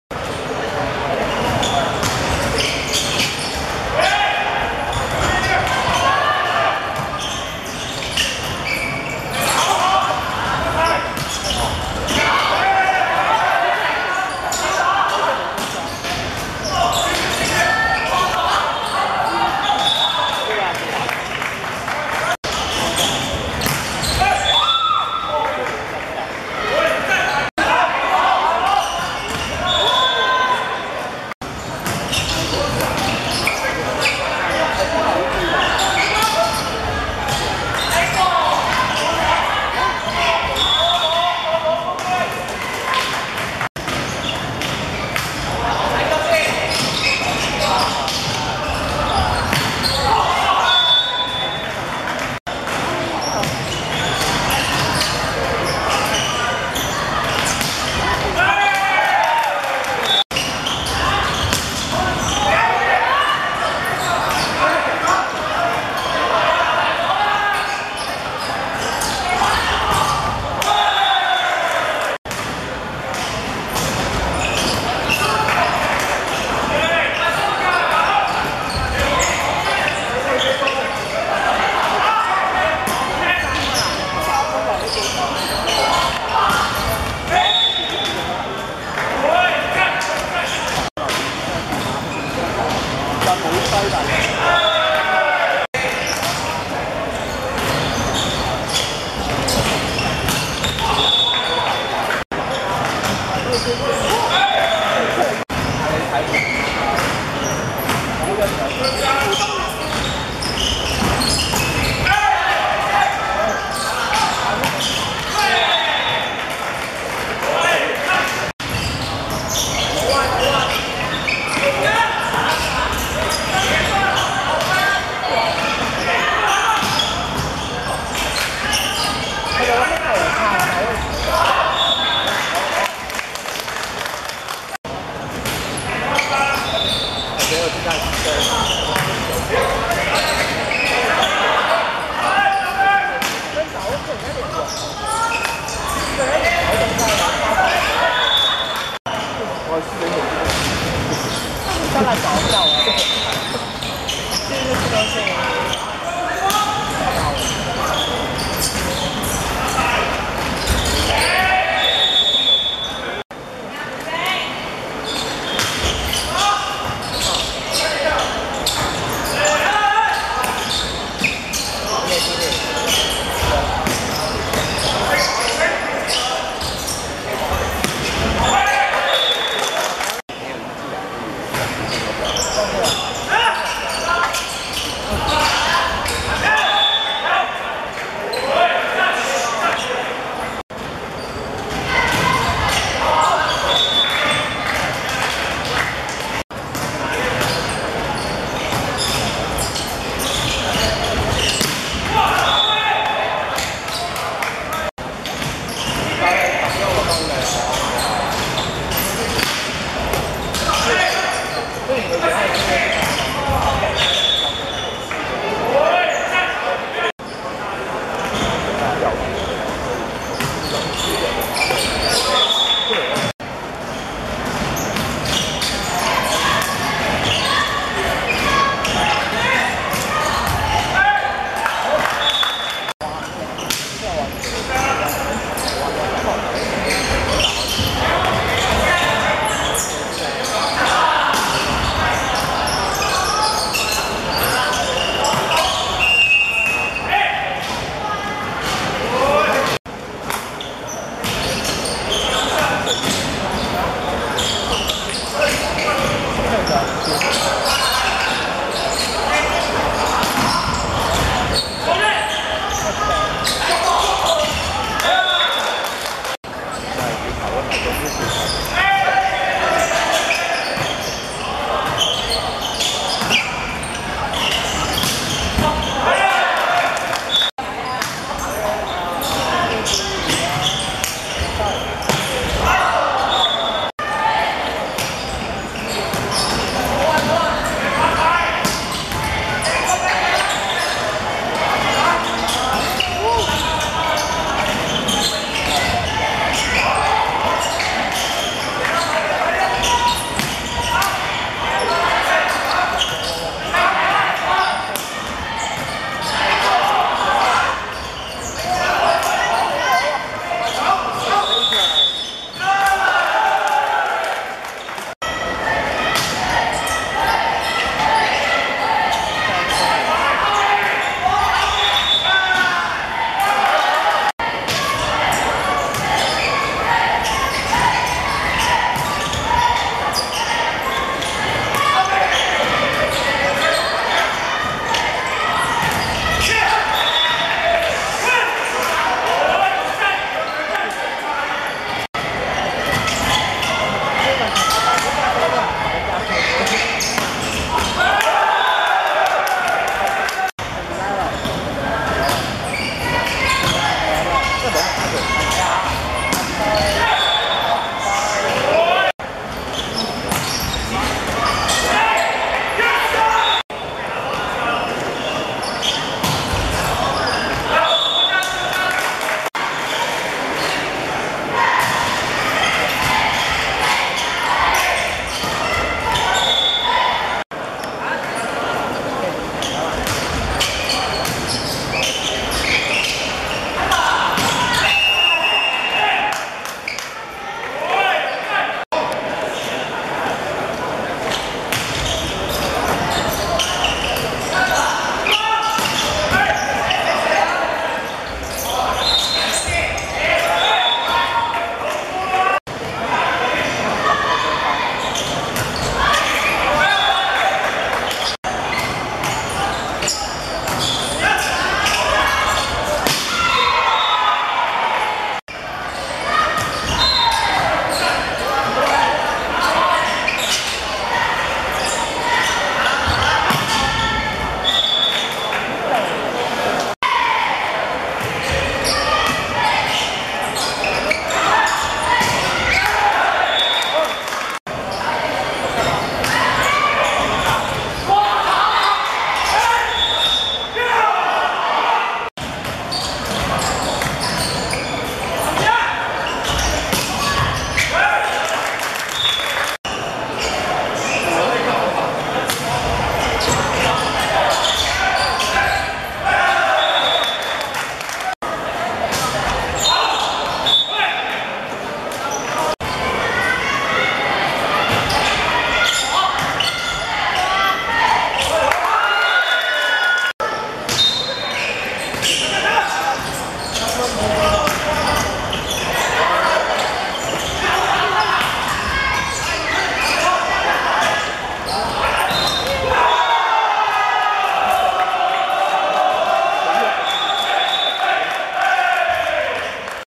仁濟二中vs培正(2012.12.29.學界男子排球精英賽八強)第二局精華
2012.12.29.青衣體育館 10:20 仁濟醫院第二中學(彩色衫)第一局25:14 第二局25:22 勝出 晋身四強